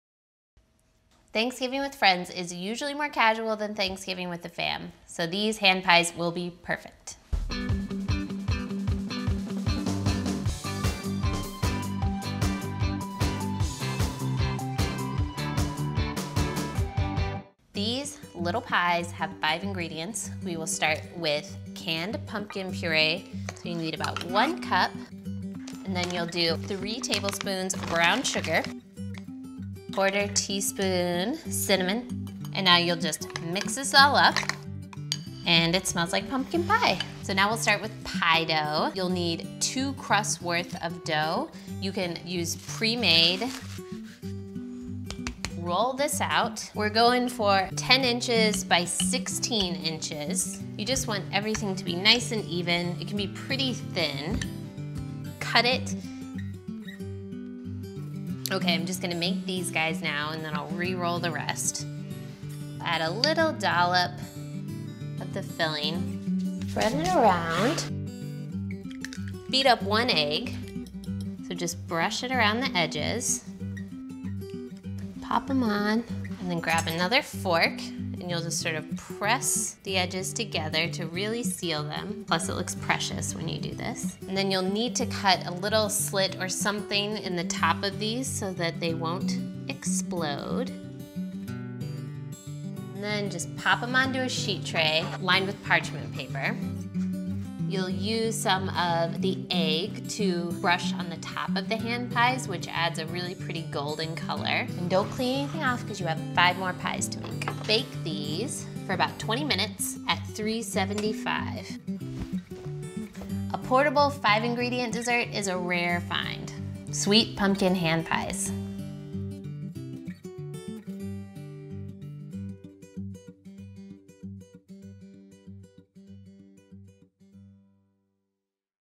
Sweet Pumpkin Hand Pies | Freshly Made | Whole Foods Market
Friendsgiving is a little more casual than Thanksgiving with the fam, so this no-forks-necessary treat is destined for dessert greatness at your friendly feast. These rectangular desserts are filled with pumpkin purée, cinnamon and brown sugar for that scrumptious pumpkin pie flavor we all look forward to this time of year. Enjoy!

Makes 10

Ingredients:
All-purpose flour for dusting
2 recipes prepared piecrust or 1 (15-ounce) package 9-inch roll-out piecrusts
3/4 cup pumpkin purée
2 tablespoons firmly packed light brown sugar
1/4 teaspoon ground cinnamon
1 egg, beaten

Method:
Preheat the oven to 375°F. Line 2 large baking sheets with parchment paper.

On a floured surface, roll out dough about 1/8 inch thick. Using a floured 3-inch round cookie cutter, cut out 20 circles, rerolling dough as needed. Place 10 circles on each of the prepared baking sheets. Using a very sharp paring knife, cut out jack-o’-lantern faces in half of the dough circles. (Alternatively, roll out each portion of the dough into 10x16-inch rectangles and cut out 10 rectangles from each, rerolling dough as needed. If making these larger pies, bump the pumpkin purée measurement to 1 cup and brown sugar to 3 tablespoons.)

In a medium bowl, stir together pumpkin purée, brown sugar and cinnamon until well blended.

Working on the parchment-paper-lined baking sheet, spoon a heaping tablespoon of pumpkin filling into center of an uncut dough circle; brush edges with beaten egg. Top with a jack-o’-lantern face circle and press edges to seal. Repeat with remaining filling and dough circles. Brush tops with remaining egg.

Bake until golden brown, about 20 minutes. Let cool slightly on the baking sheets before serving. Hand pies will keep in an airtight container at room temperature for up to 3 days.

Nutritional Info:
Per Serving: Serving size: 1 pie, 210 calories (100 from fat), 12g total fat, 4g saturated fat, 20mg cholesterol, 200mg sodium, 23g carbohydrates, (1 g dietary fiber, 5g sugar), 2g protein.